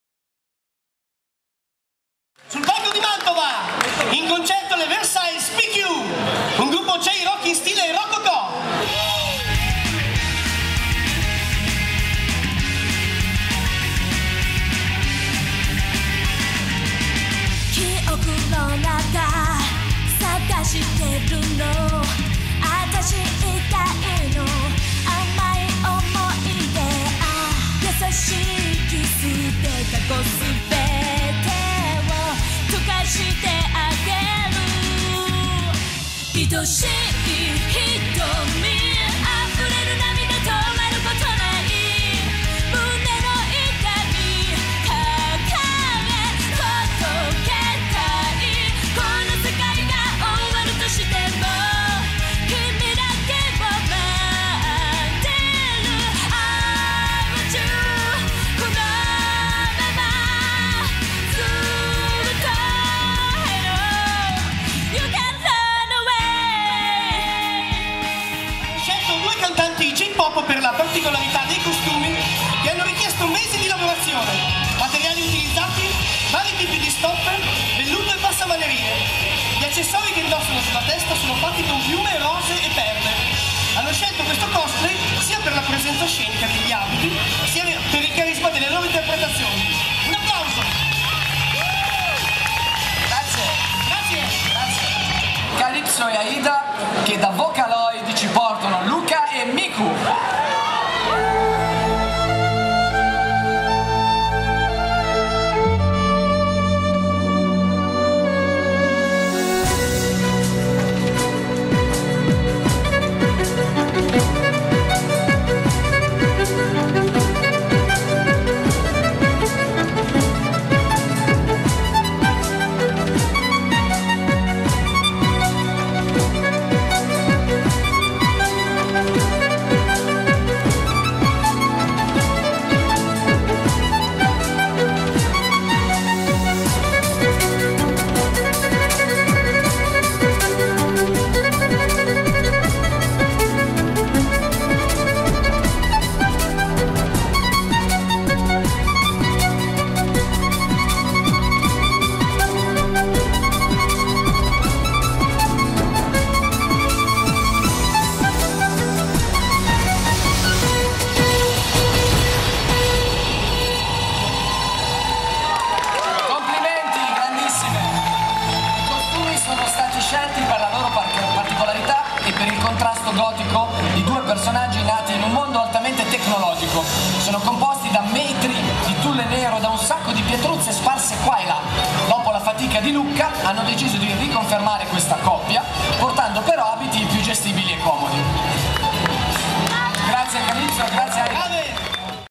COSPLAY EVOLUTION 2011: video ufficiali parte 3
Sul palco di Mantova Comics: le bellissime esibizioni delle Versailles PQ e delle Vocaloid!
Riprese di Giorgio Tornieri.